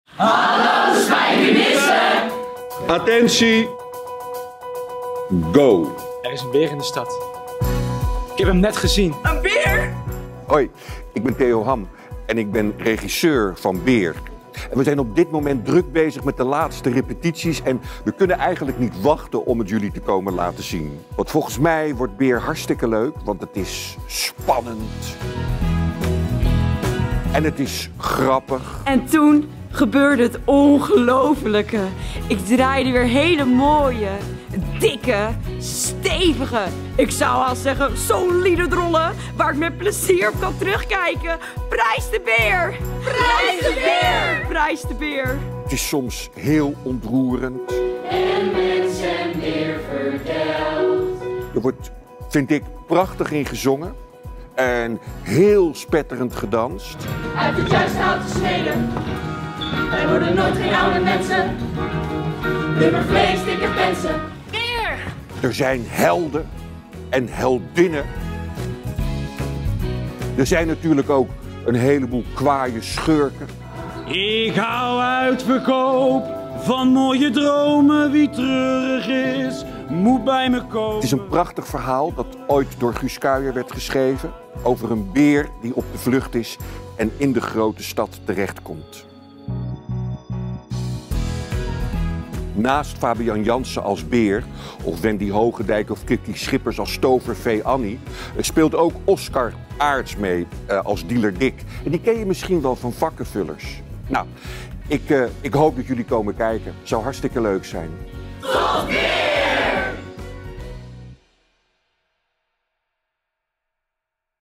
BEER spijkenisse
Dit theaterseizoen komt beer naar de grote stad, wat uitloopt in een doldwaas, muzikaal avontuur voor het hele gezin. Beer is groot, zacht en lief. Maar toch, die klauwen... Mag beer blijven, of moet hij gevangen en uitgezet worden? 7 december 2019 te zien in De Stoep in Spijkenisse.